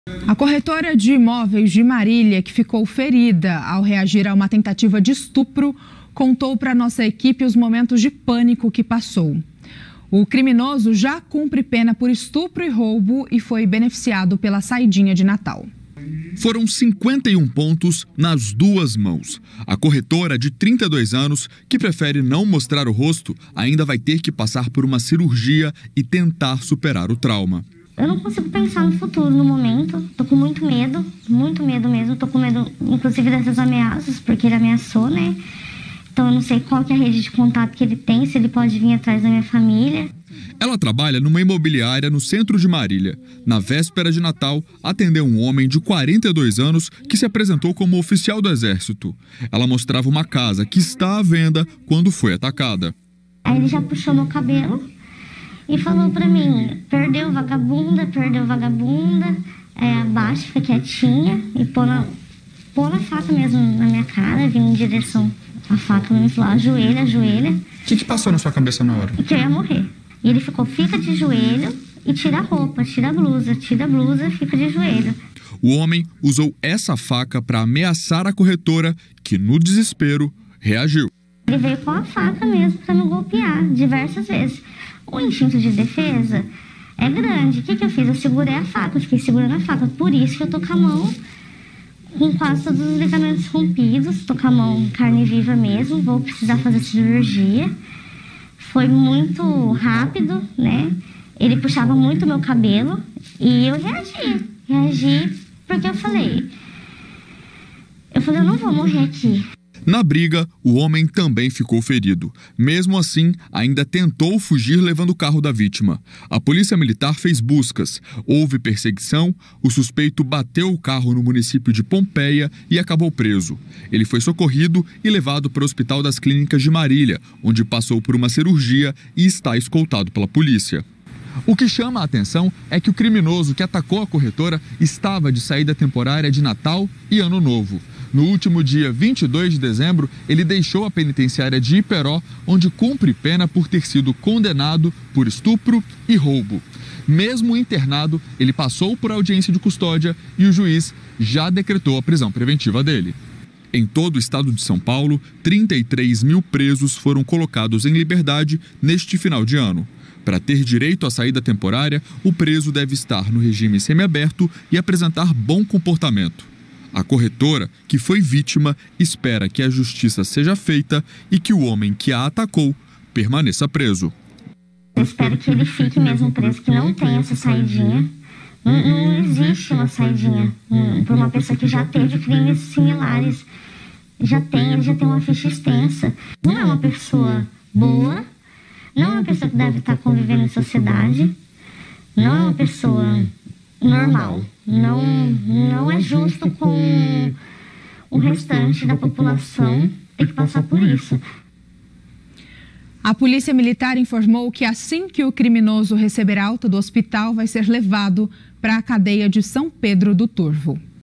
PRESO BENEFICIADO PELA SAIDINHA DE NATAL E ANO NOVO ATACA MULHER TENTANDO ESTUPRÁ-LA
Preso beneficiado pela saidinha de Natal e ano novo ataca mulher tentando estuprá-la. Detalhe: ele havia sido preso por roubo e ESTUPRO. É inadmissível que tenhamos vagabundos presos por crimes como esse recebendo esse benefício.